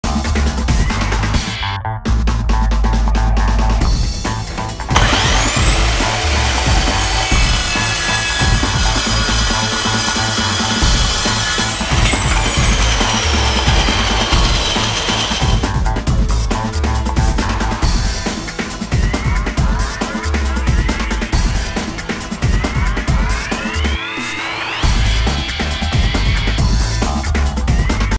Steelmax - S9 9" Metal Cutting Saw
The most powerful saw of this size in the market, the Steelmax S9 is designed for heavy duty industrial uses and widespread on-site applications including: fabrication, construction, and fire and rescue. Competitively priced, the S9 cuts mild steel, stainless steel and aluminum as easy as cutting plywood.